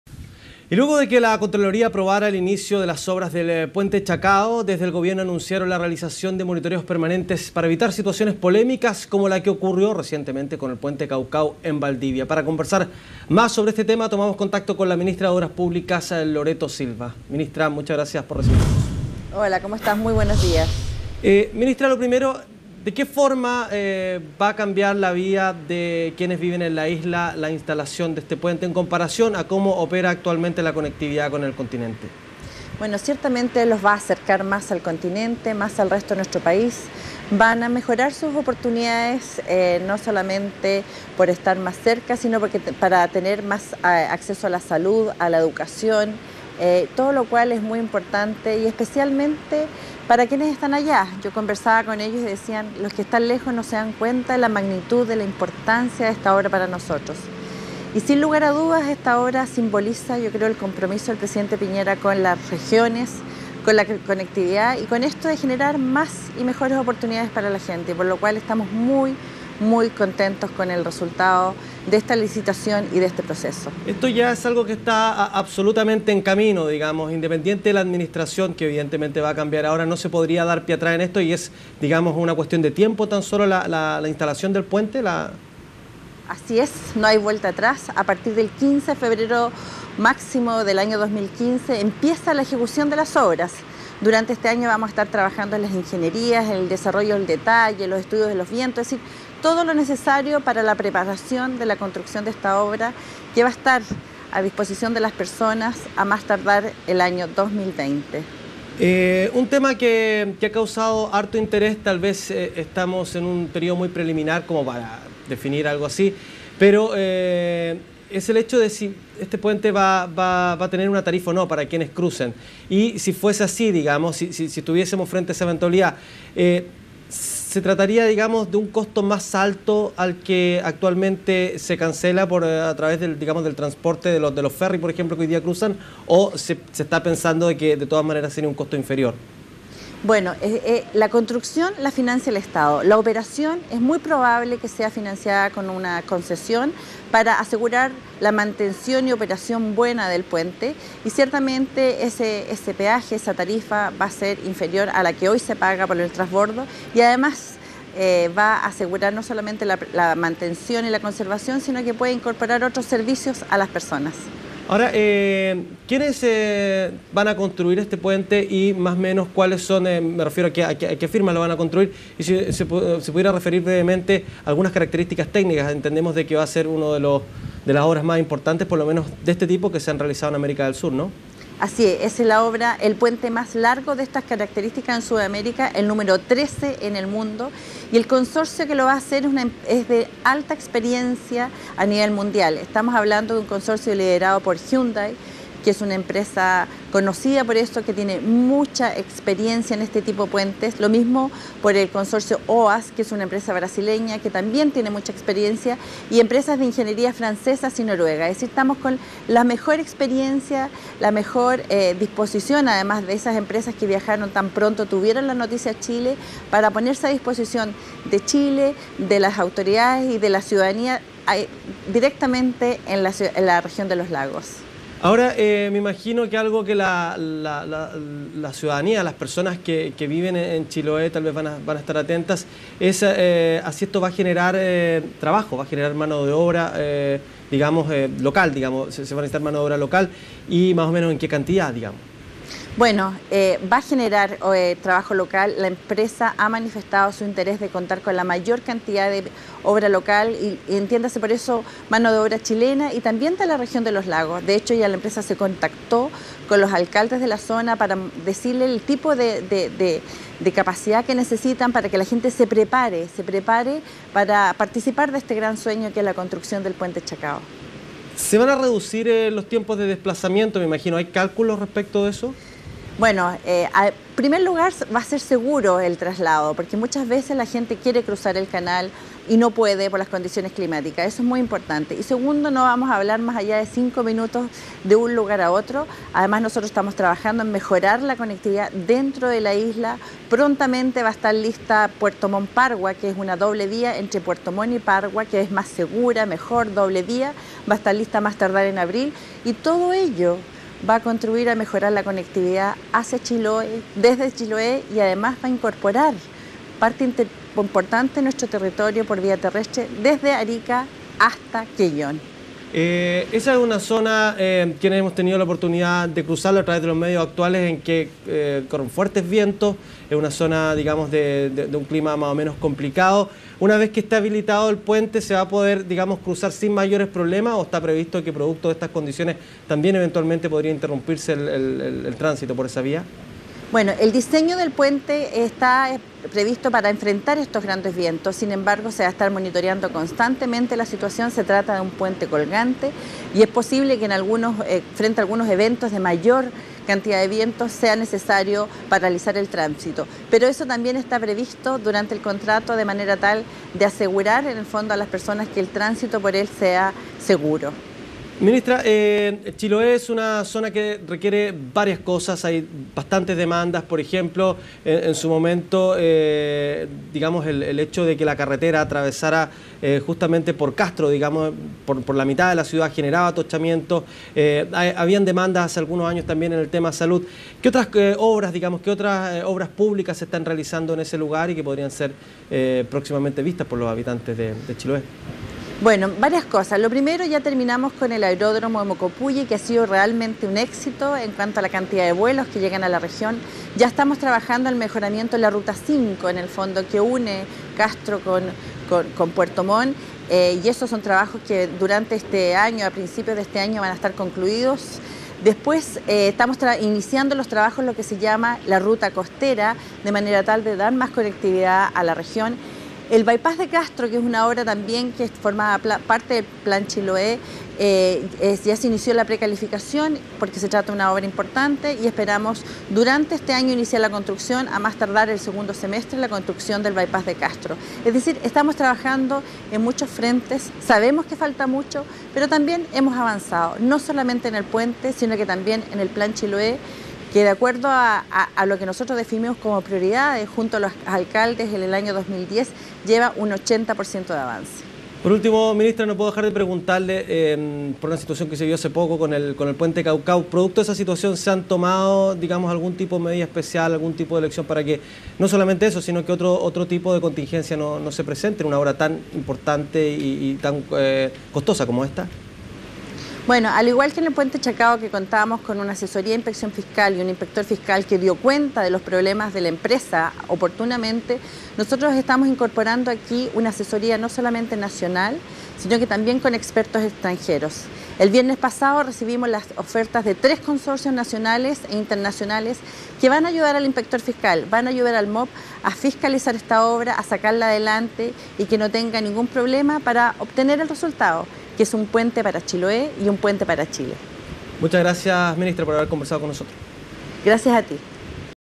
Ministra Loreto Silva se refirió a la construcción del Puente de Chacao
La obra contempla una inversión de más de 360 mil millones de pesos y medirá 2.750 metros.